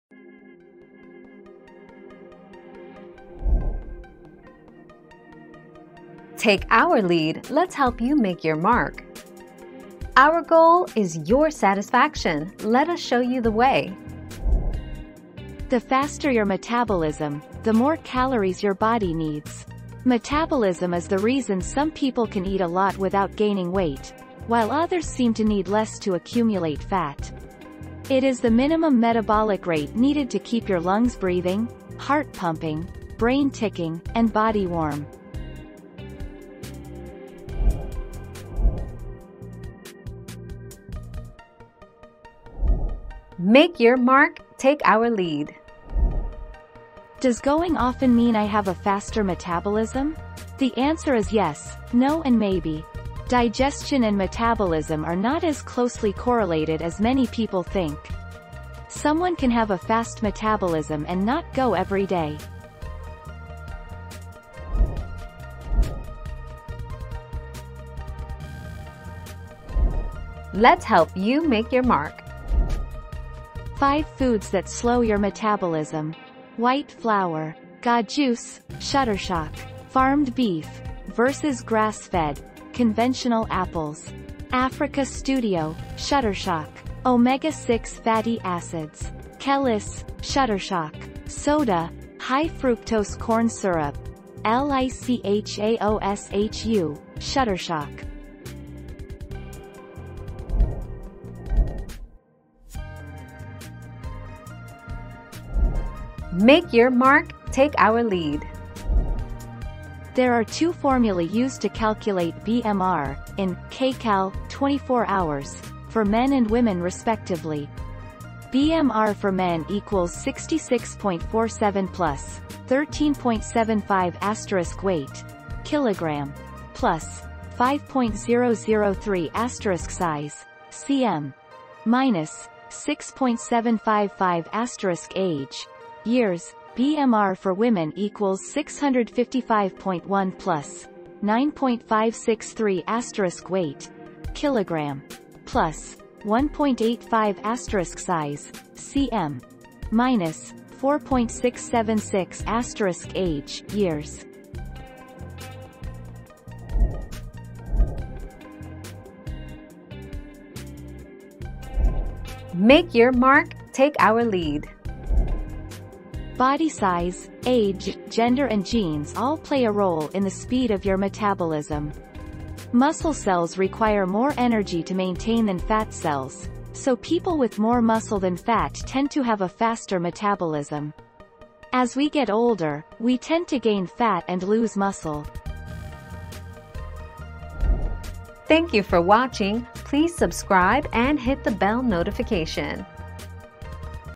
Is it good to have a fast metabolism?
00:00 - Is it good to have a fast metabolism?
00:39 - Do I have a fast metabolism if I poop a lot?
01:06 - What foods slow down your metabolism?
01:42 - How do I know what my metabolism is?
02:38 - What causes fast metabolism?

Laura S. Harris (2021, June 9.) Is it good to have a fast metabolism?
    AskAbout.video/articles/Is-it-good-to-have-a-fast-metabolism-252111